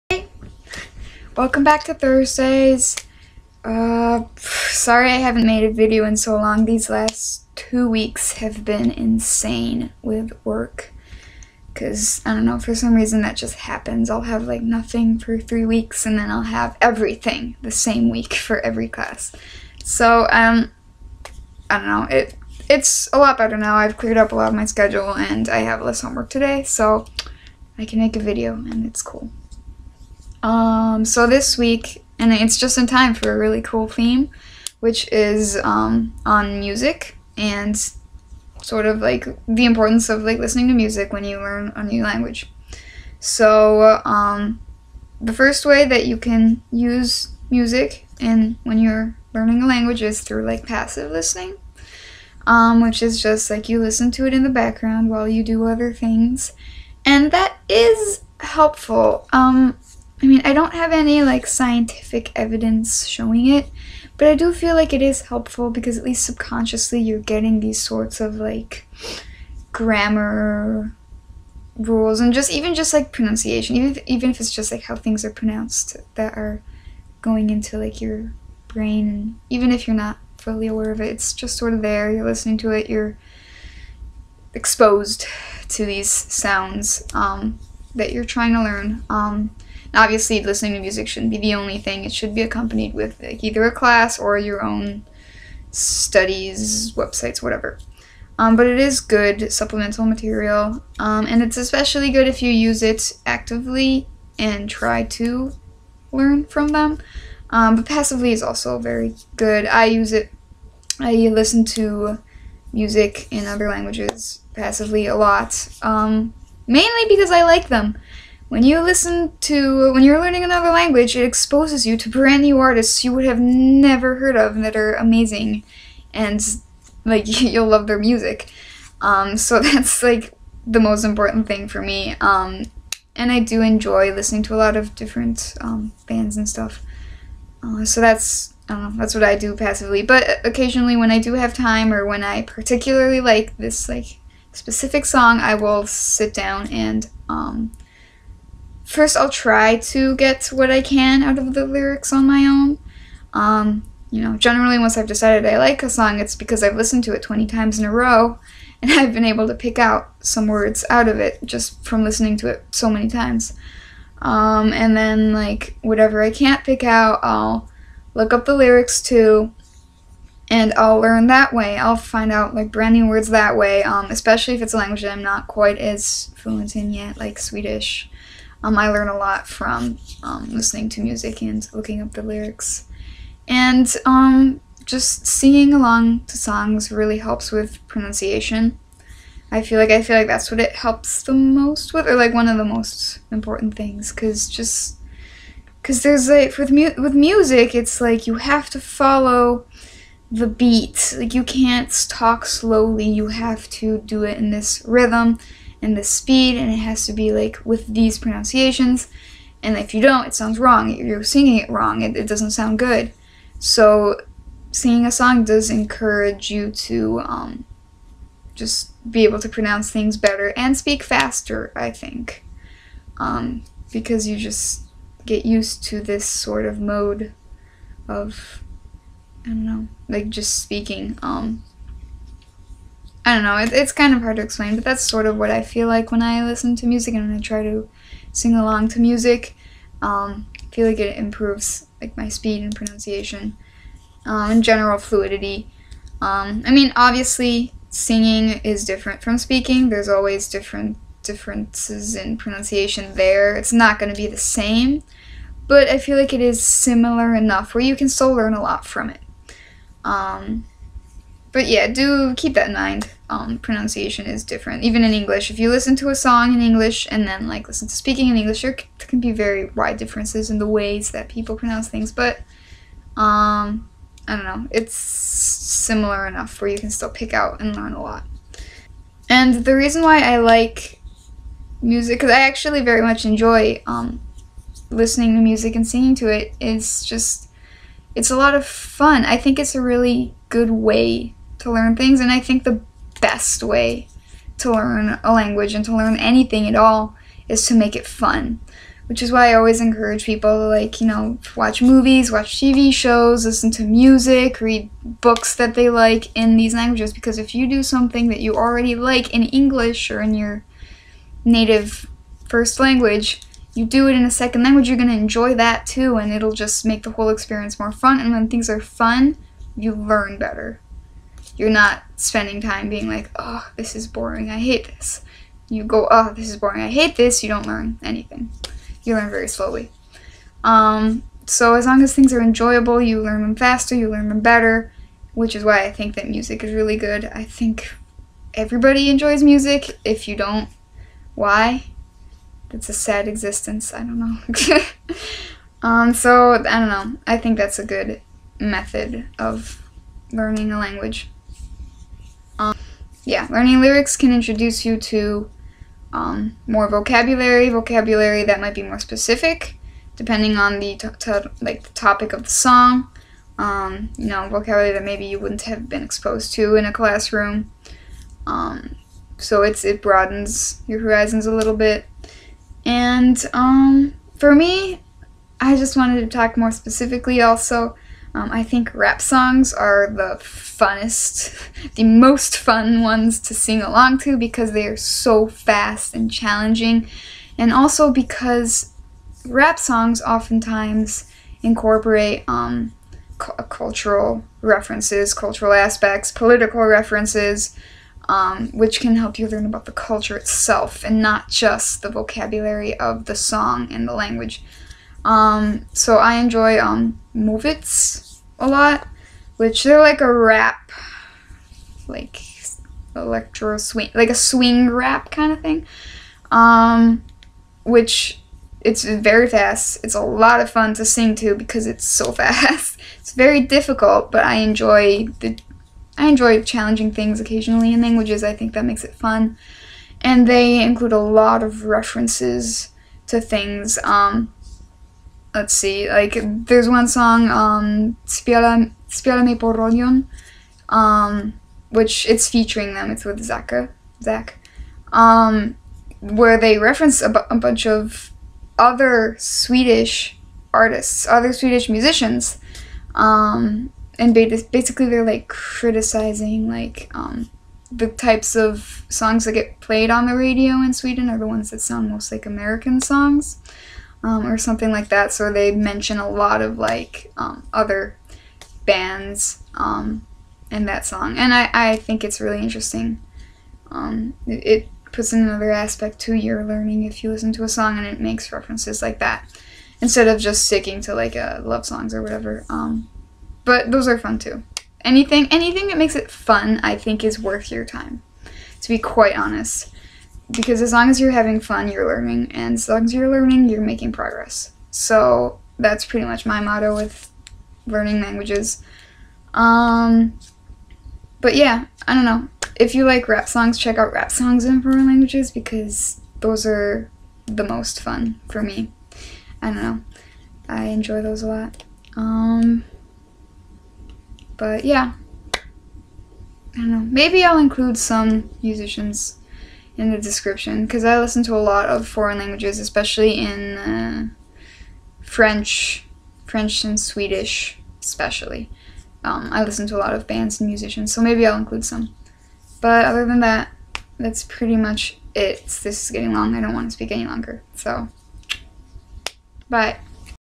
Music and Language Learning
Bands to listen to:

French:
Stromae (Belgian)
Mika has at least one French song
Grand Corps Malade (rap!)
Really there's a lot of French songs, you shouldn't have an trouble finding them. I'll add more if I think of them, of course I blank out now

Movits!
Timbuktu
Kent
Stiftelsen
Den Svenska Björnstammen
Basshunter has a few Swedish songs